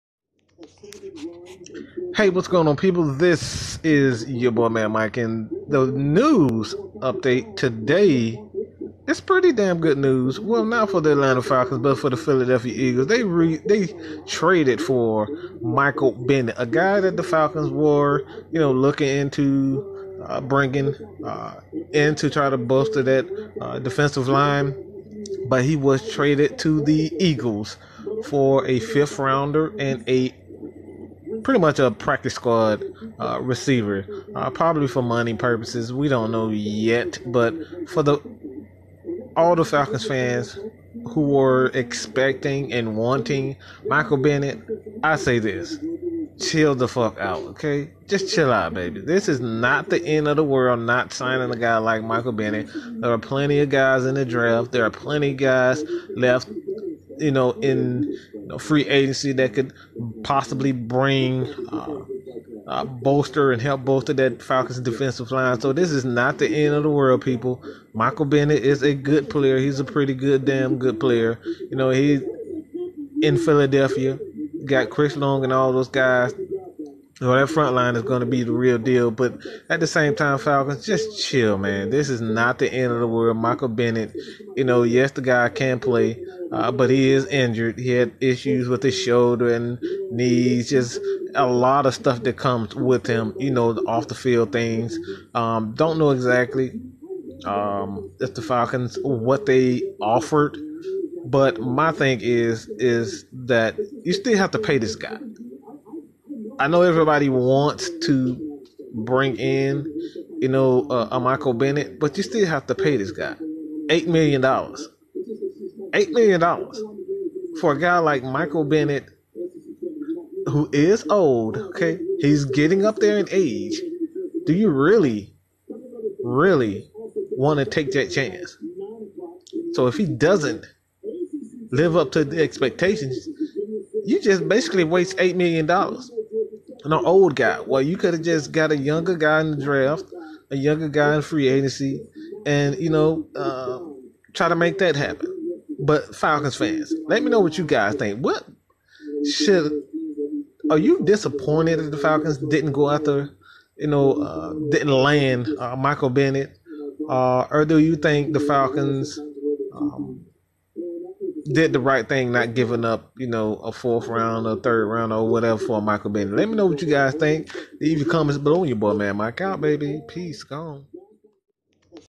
Michael Bennett Traded To Philadelphia Eagles
Falcons miss out on Michael Bennett trade. Seattle Seahawks trade defensive end to Philadelphia Eagles for 5th round.

Mad Mike gives his take on the matter.

Are you upset about Falcons not landing Bennett?